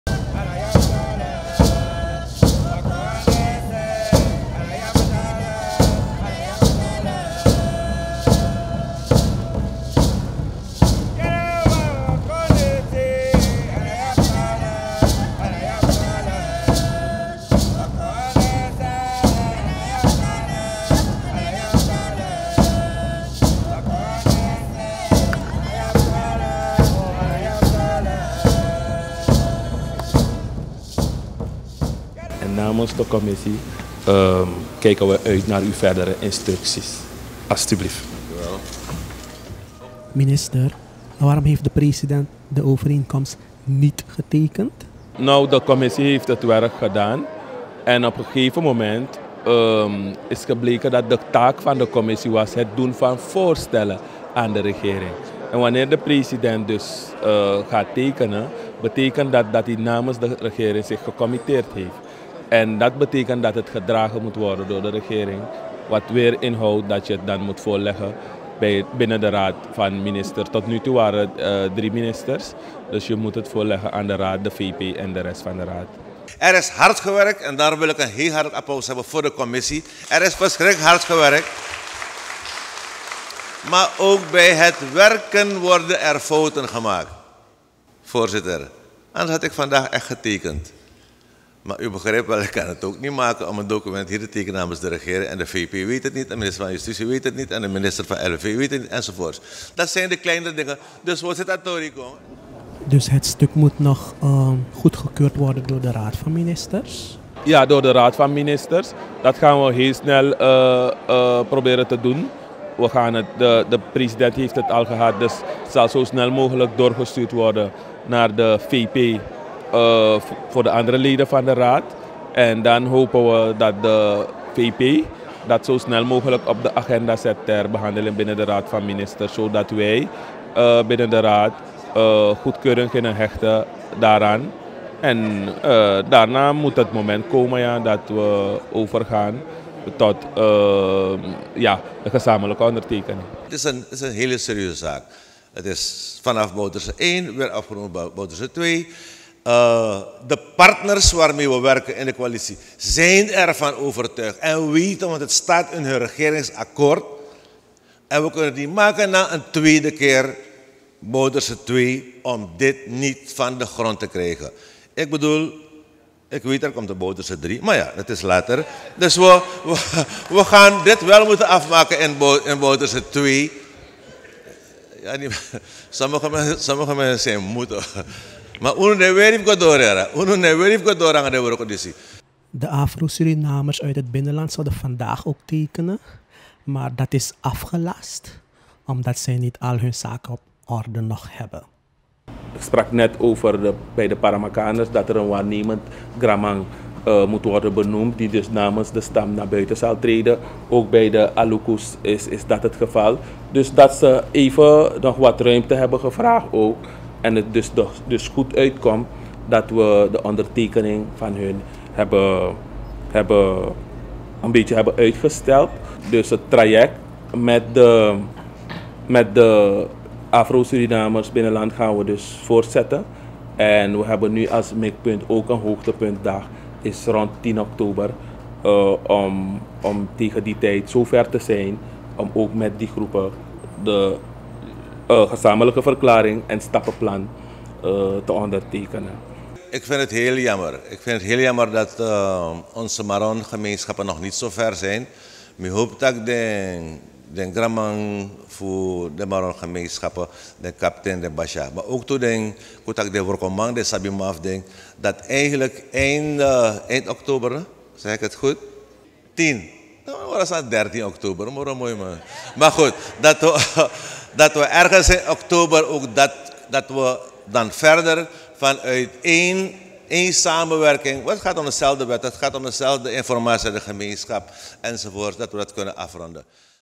President tekent gezamenlijke verklaring met Inheemsen niet
President Desi Bouterse heeft de ‘gezamenlijke verklaring’ met de Inheemsen om het grondenrechtenvraagstuk op te lossen, niet getekend. De regering moet de punten en stappen opgenomen in de overeenkomst eerst nog goedkeuren. De ondertekening met de Afro Surinamers uit het binnenland, die ook voor vandaag was gepland, is een paar dagen geleden afgeblazen.